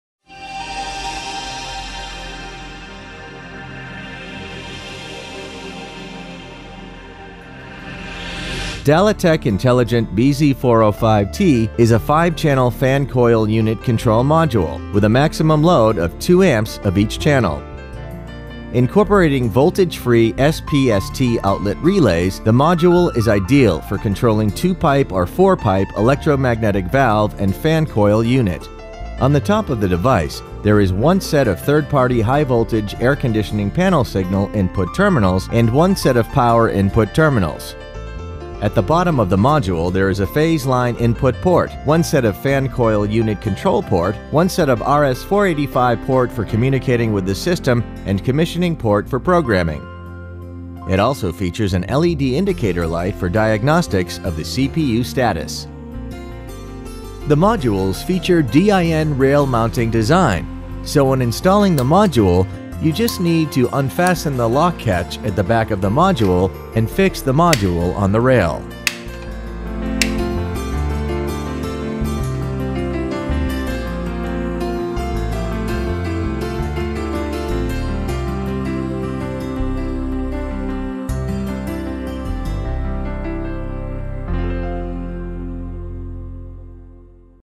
BZ405T Fan Coil Unit Control Module
Bridges' intelligent BZ405T fan coil unit control module is designed to control
two-pipe or four-pipe solenoid valves and fan coil units. The module can be controlled with the BX400HU Network interface and BZ-HU-T sensor for intelligent operation and is DLNet bus enabled.